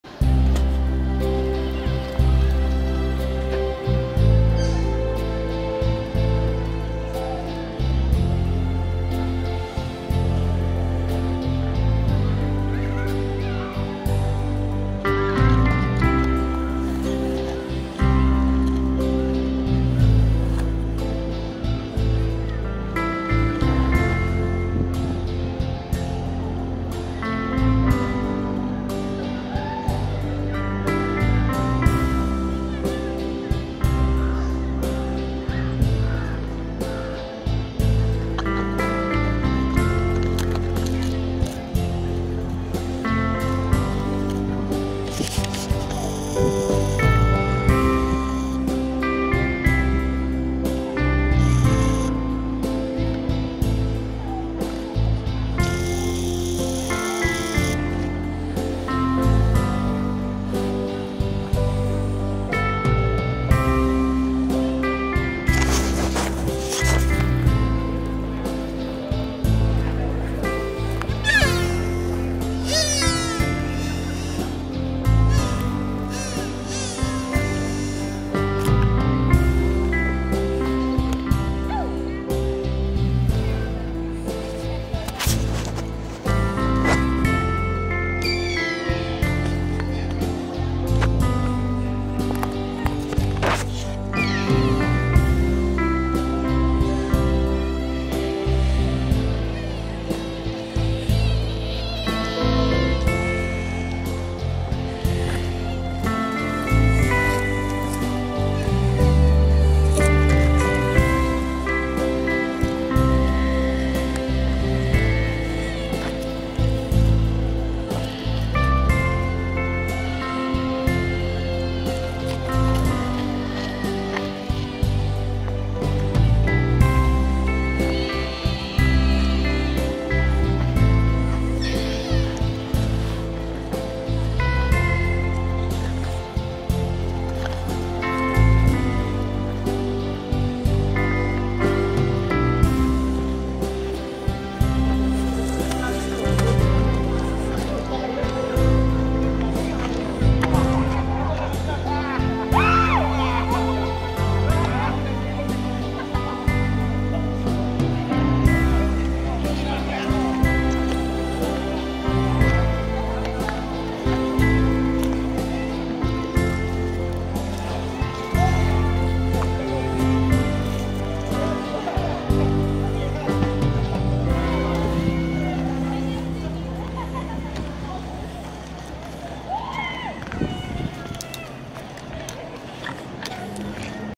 Feeding adorable Deer at Nara Park2 かわいい鹿にせんべいを与える:奈良公園２
At Nara Park in Japan, visitors can purchase "deer-crackers" (鹿煎餅 Shika-senbei) to feed the deer in the park.
This video shows what it is like to feed deer with the crackers.
Hungry deer will flock to you to demand crackers.